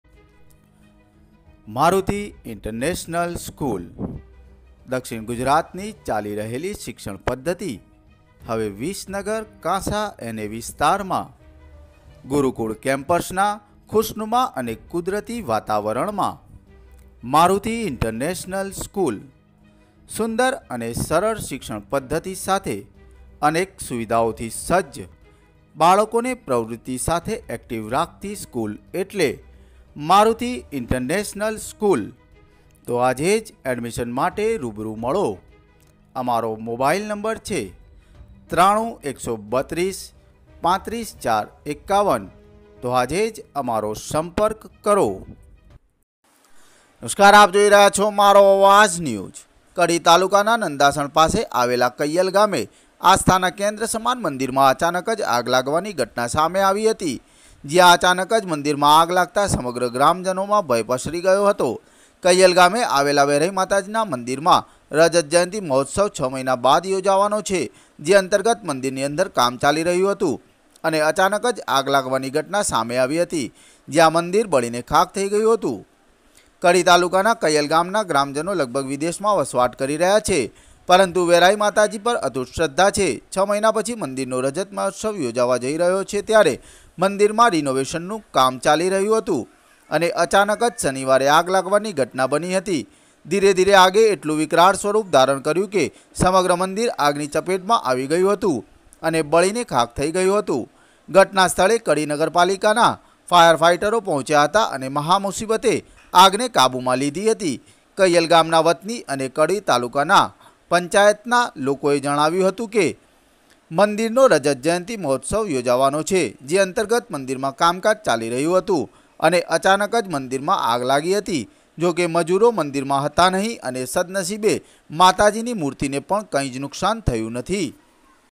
કડી:કૈયલ ગામના મંદિરમાં આગ લાગી મંદિર બળી ખાક..
કડીના કૈયલ ગામે આગ લાગેલી છે મંદિરમાં આખું મંદિર બળી ગયેલ છે મૂર્તિને કઈ થયું નથી અને અને બીજી કોઈ જાનહાની થઈ નથી